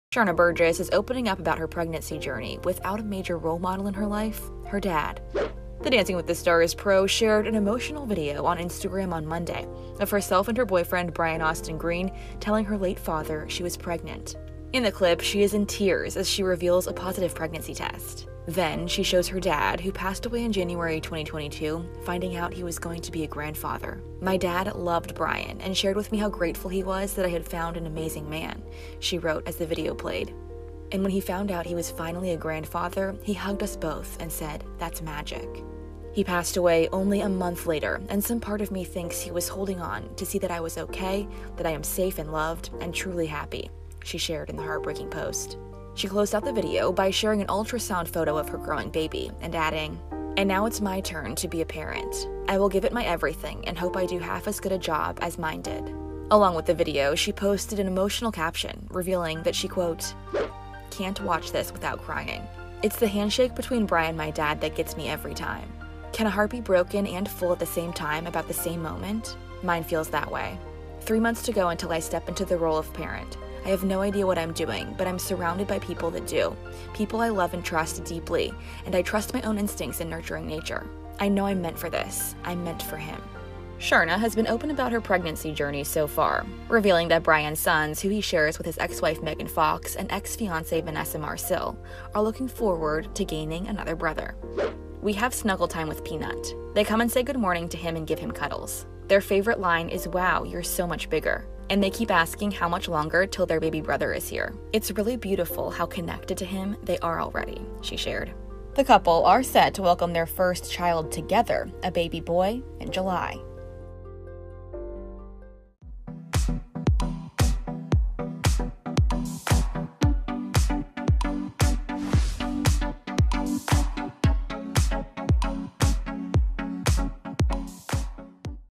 Sharna Burgess Reveals Emotional Moment Her Late Father Found Out She Was Pregnant
Sharna Burgess is opening up about her pregnancy journey without her father. The "DWTS" pro shared an emotional video on Monday of herself and her boyfriend, Brian Austin Green, telling her father she was pregnant before he passed away. "When he found out he was finally a grandfather, he hugged us both and said, "that's magic ... He passed away only a month later and some part of me thinks he was holding on to see that I was ok. That I am safe and loved and truly happy," she shared.